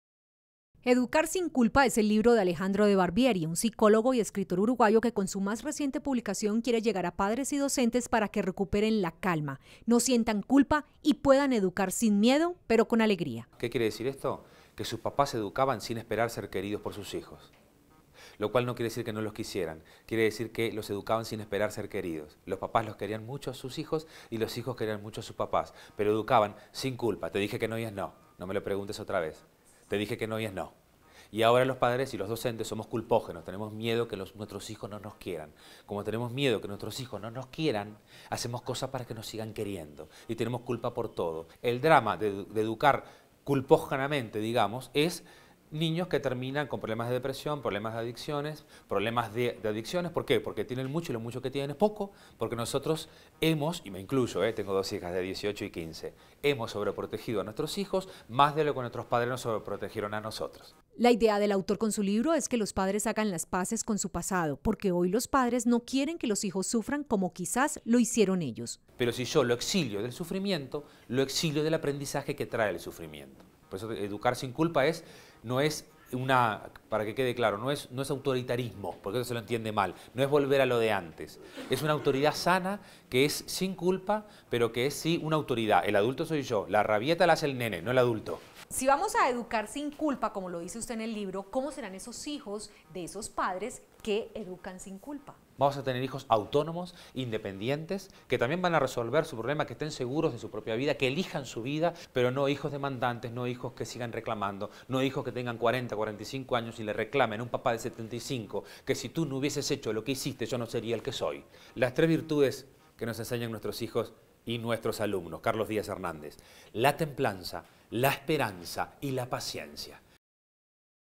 LOS PADRES DEBEN APRENDER A EDUCAR SIN CULPA
EDUCAR SIN CULPA ES EL LIBRO DE ALEJANDRO DE BARBIERI, UN SICOLOGO Y ESCRITOR URUGUAYO QUE CON SU MAS RECIENTE PUBLICACION QUIERE LLEGAR A PADRES Y DOCENTES PARA QUE RECUPEREN LA CALMA, NO SIENTAN CULPA Y PUEDAN EDUCAR SIN MIEDO PERO CON ALEGRIA